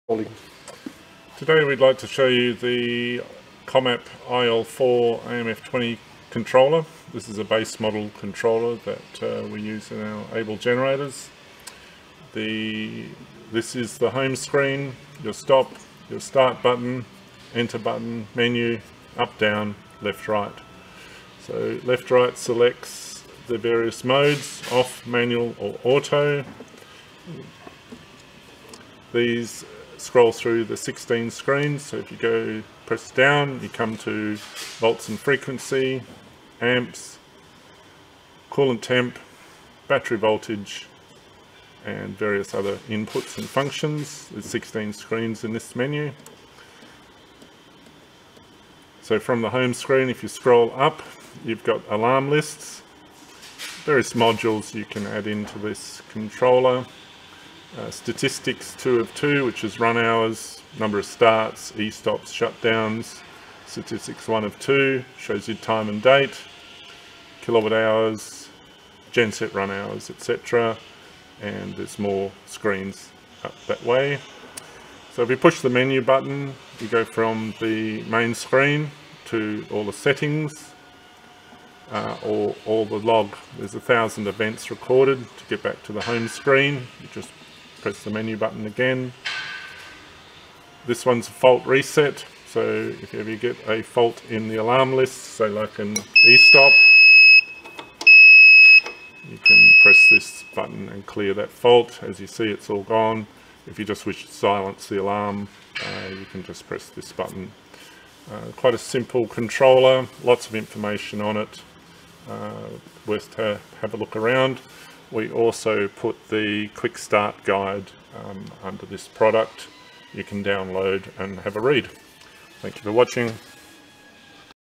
Comap AMF20
A demonstration of Comap IL4 AMF20 diesel generator controller